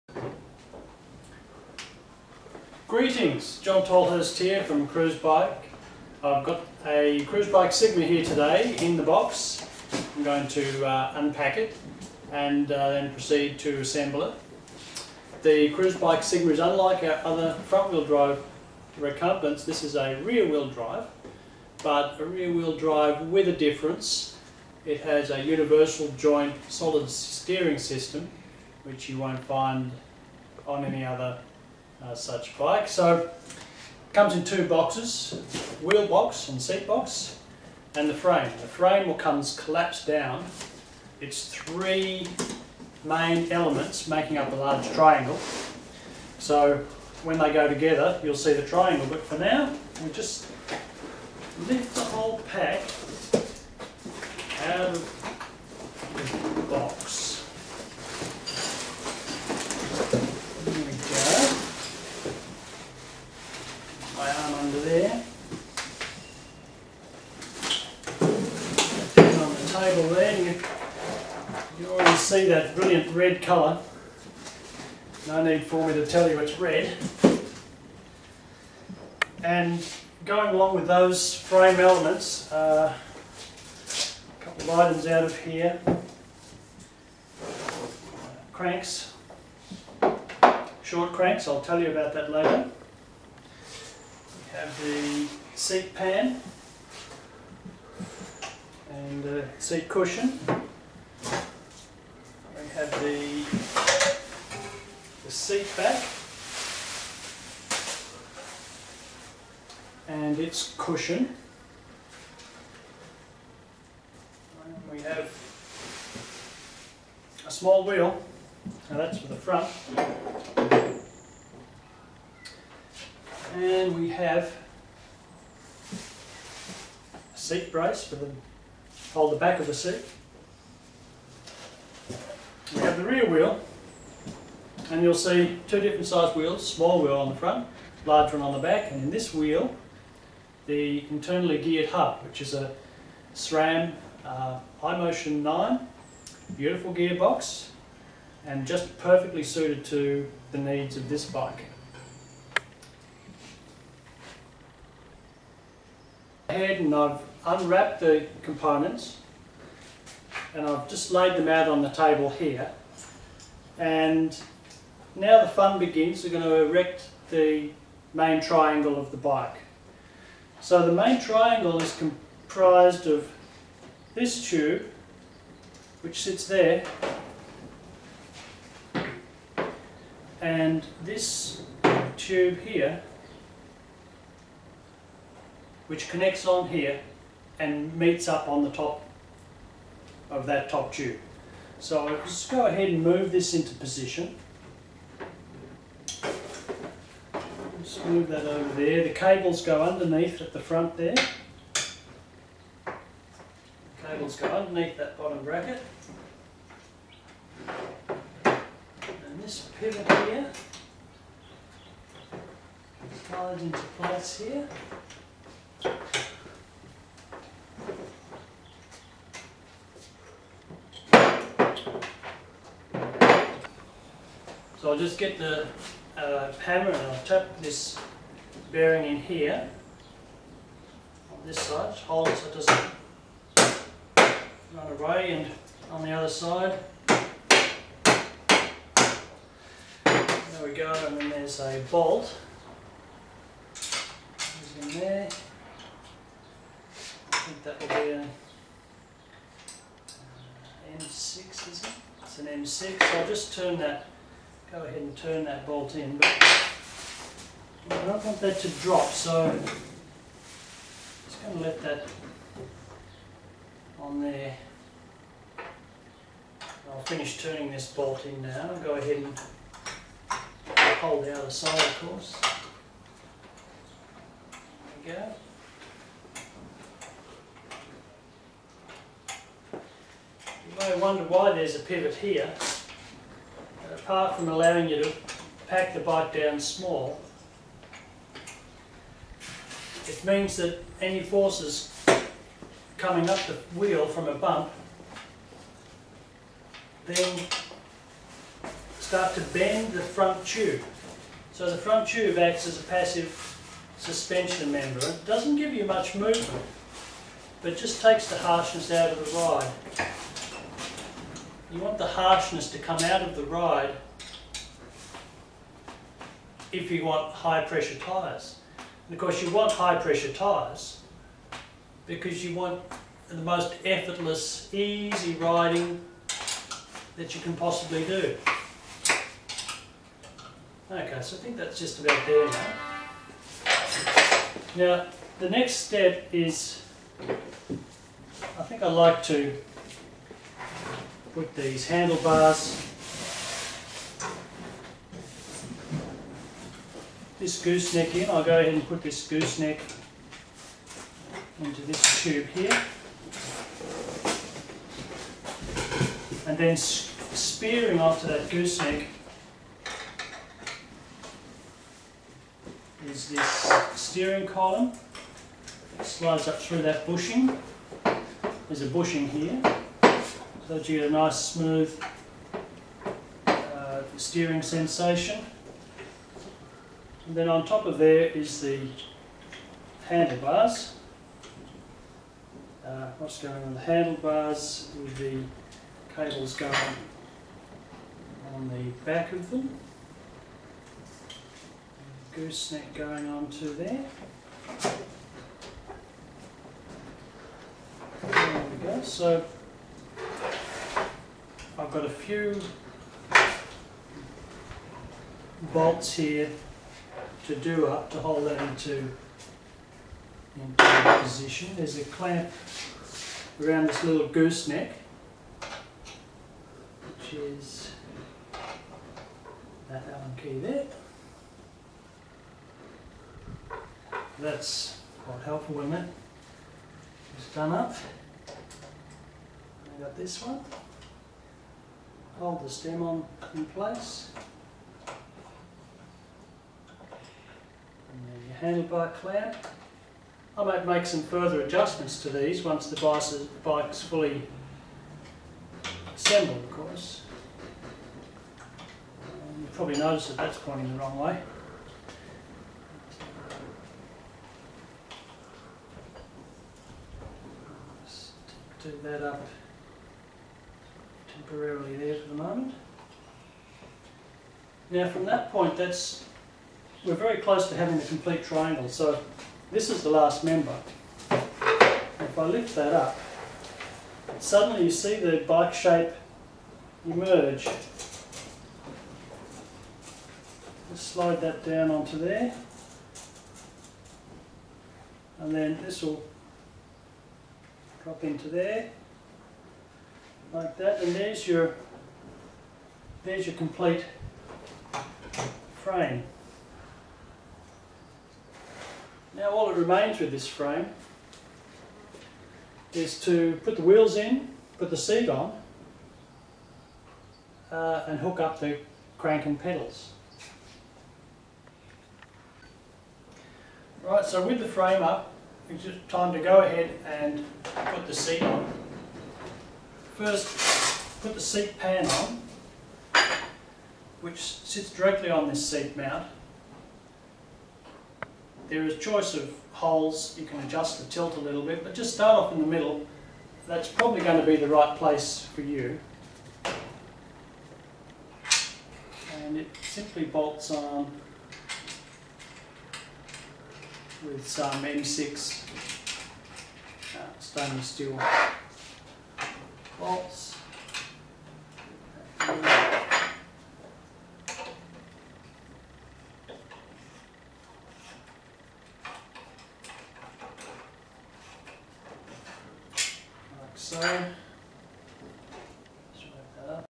Sigma Assembly, Part I
John Tolhurst, Design Director of Cruzbike Inc assembles a Cruzbike Sigma. This is a demonstration done on one of the intial short production runs of this bike.